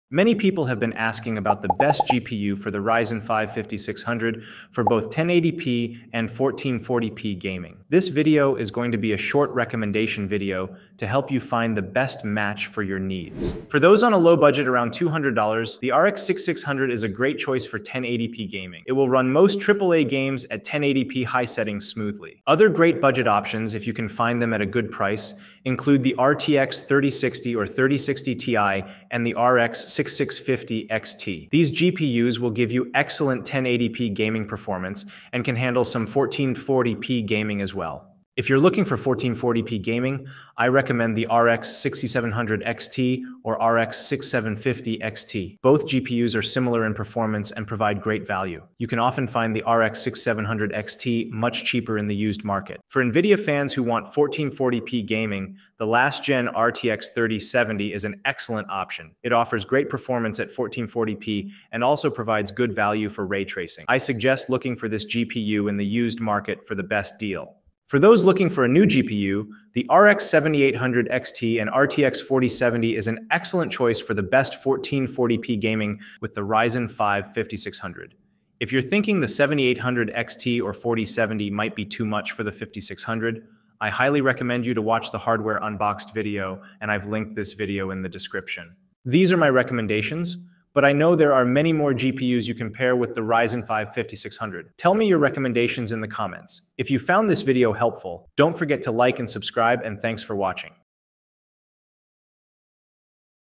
Best GPU for Ryzen 5 5600
Best GPU for Ryzen 5 5600 in 2025

RX 6600
RX 6650 XT
RTX 3060
RTX 3060 TI
RX 6750 XT
RX 7800 XT
RTX 4070

00:00 Intro
00:13 Best budget gpu for Ryzen 5 5600
00:38 Best 1440p GPU for Ryzen 5 5600
01:08 Best new GPU for Ryzen 5 5600
01:24 End

Amazon India

Camera- Fujifilm X-T100
Mics-
Boya BY-MM1

Gamers Discussion Hub